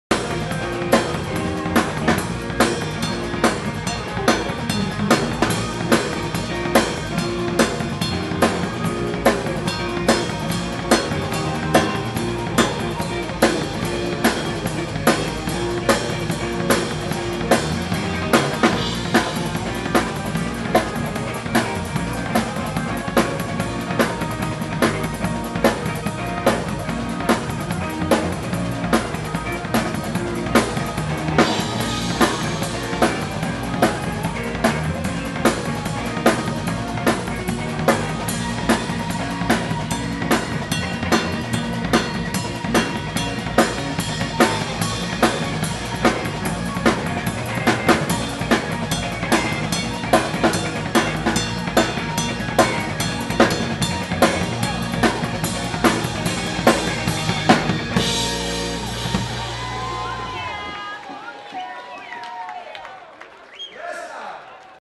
Ludwig Maple Classic - Small Club - GoPro Audio
Having a little fun with my Ludwig Classic maple kit at a show in Rocklin, California!
16x26 Bass Drum
10x14 Tom
16x16 Floor Tom
5x14 Supraphonic
Paiste Signature Reflector Cymbals
Remo Heads
Vic Firth Titan Sticks